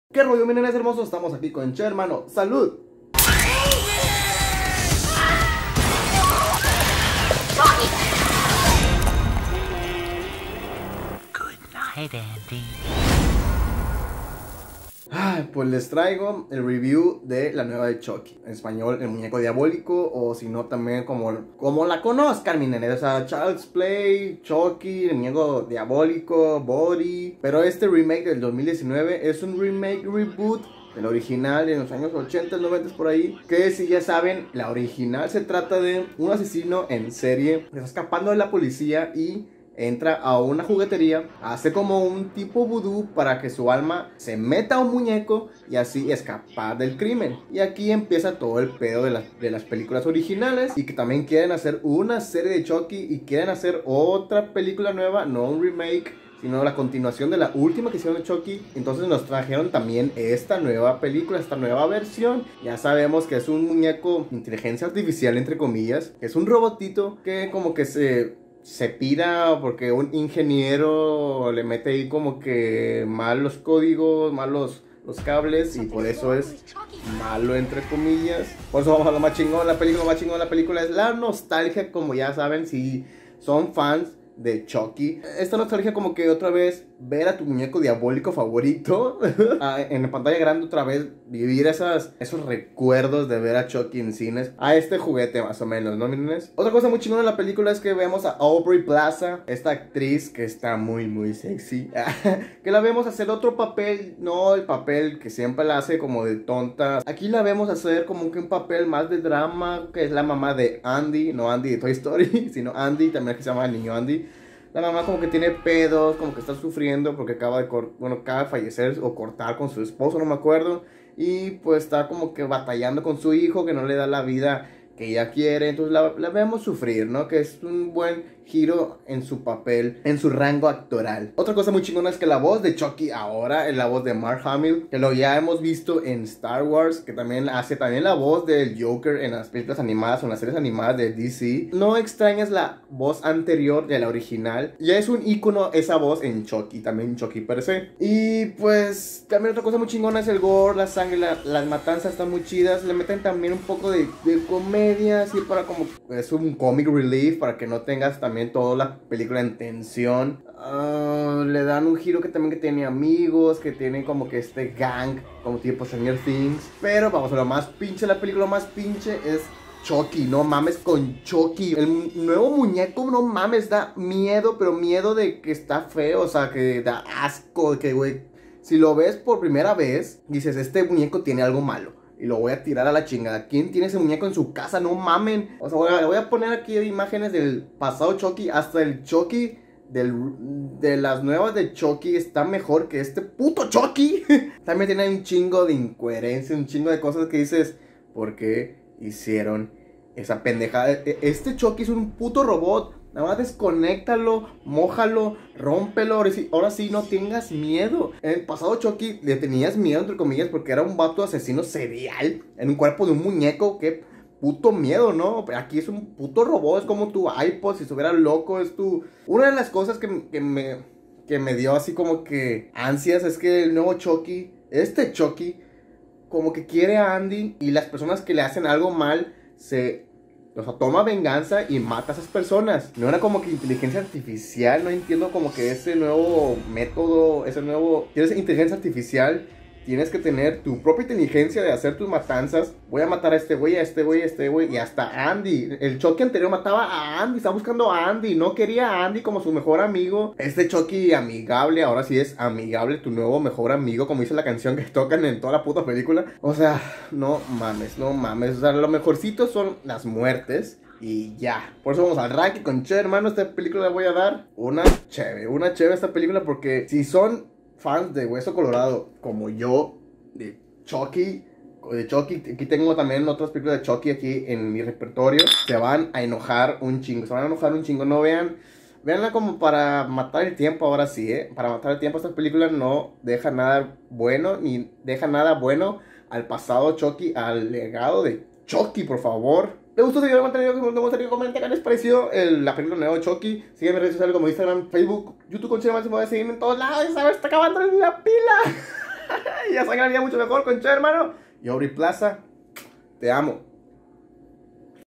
El Muñeco Diabólico (2019) // El PEOR Chucky que hemos visto!
Esta película es un remake/reboot del muñeco asesino favorito de todos, Chucky. Nos traen una historia diferente, con personajes nuevos y un chucky remasterizado pero mal, todo esta mal !!!
SALUUUUUUD!